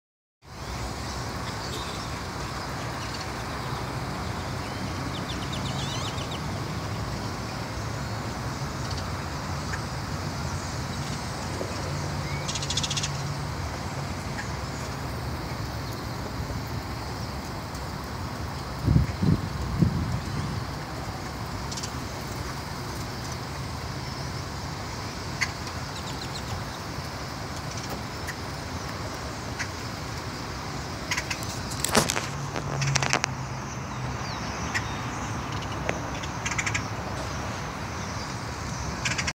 Birds migrate in Katy Texas I-10 and Fry Road
Hundreds and hundreds of birds that come to this Odd place.